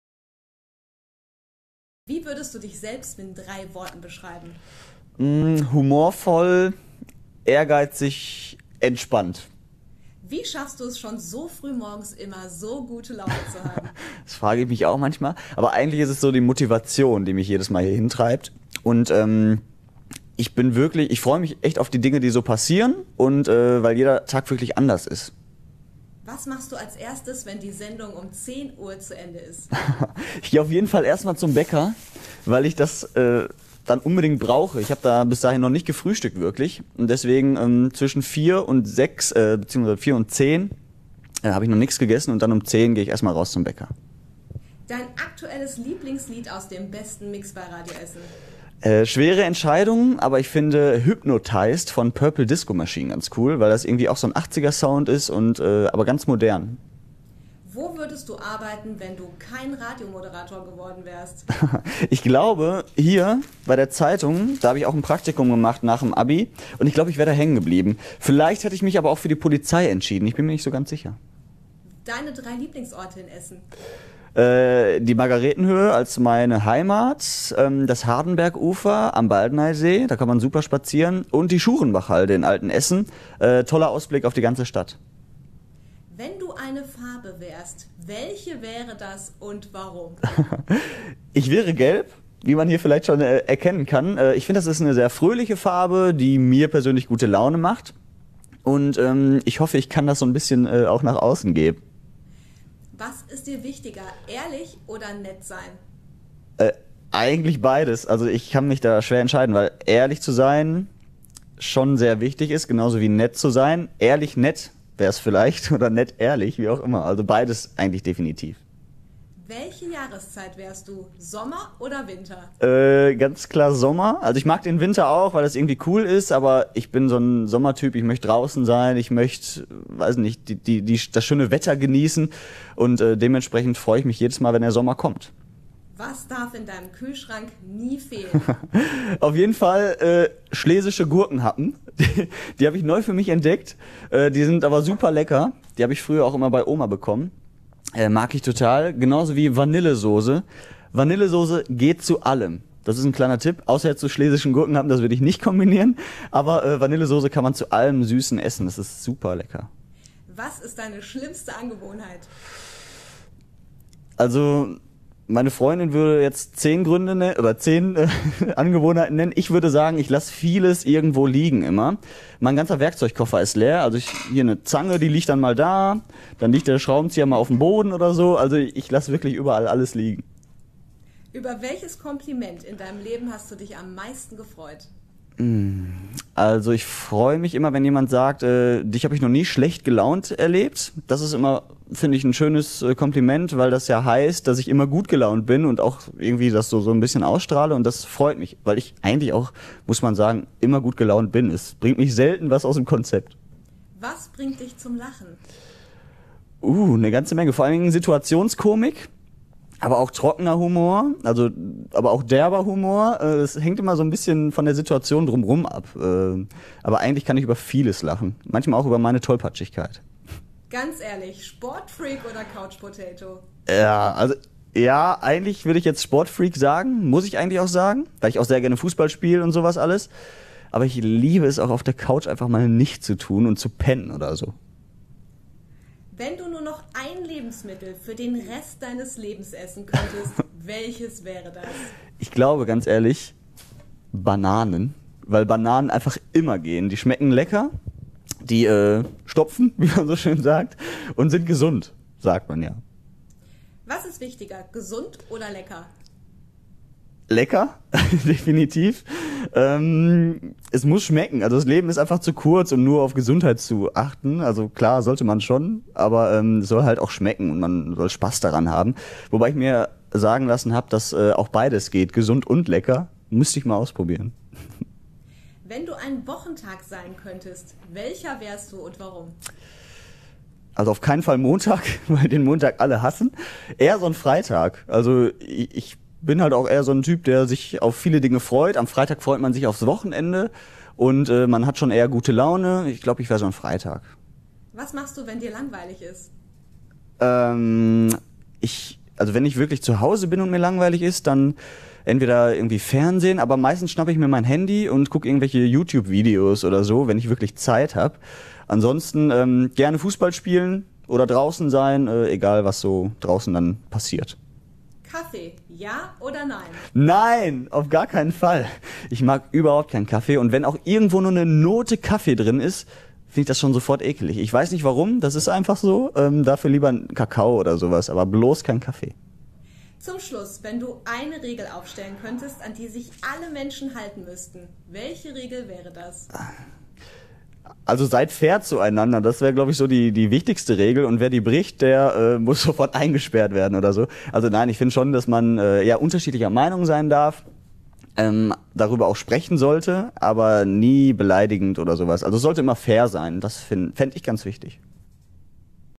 Radio Essen Am Morgen Moderator Joshua Windelschmidt im Steckbrief
Wir stellen Euch unsere Radio Essen Morgen-Teams vor mit einem Steckbrief der etwas anderen Art. Joshua Windelschmidtl beantwortet Fragen wie "Welche Jahreszeit wärst Du?" oder "Was darf nie in Deinem Kühlschrank fehlen?".